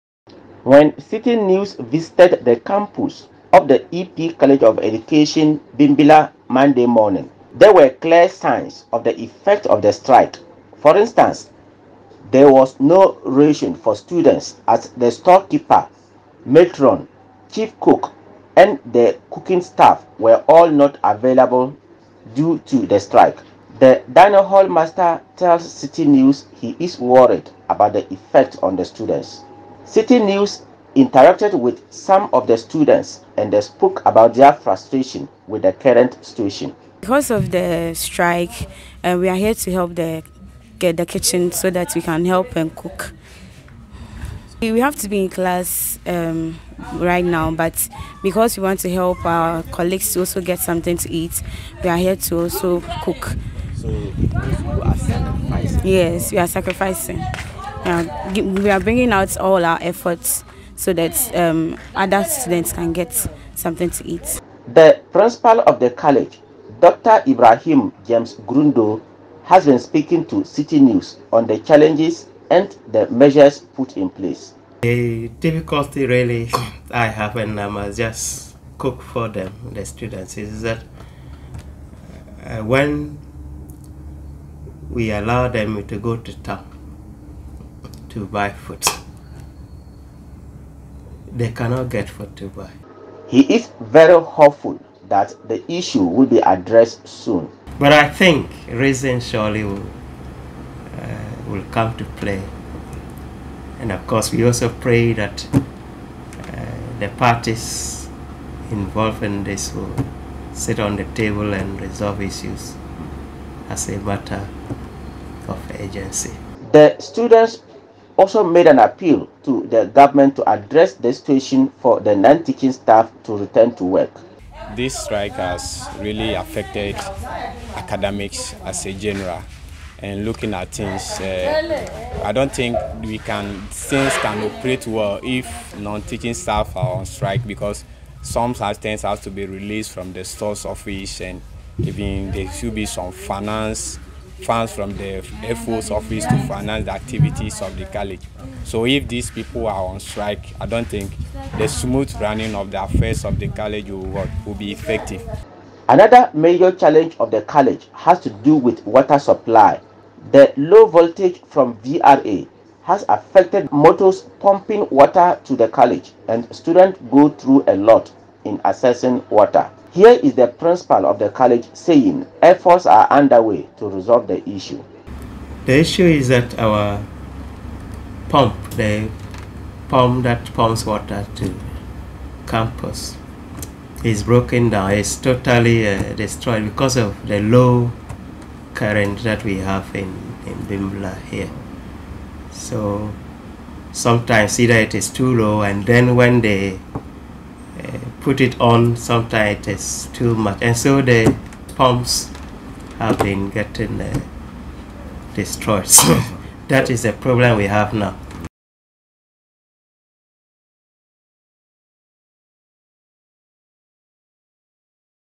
EP College of Education students bearing the brunt of non-teaching staff strike action
The evidence of the strike action of the CENTSAG is evident on how students of the EP College of Education in Bimbilla are struggling.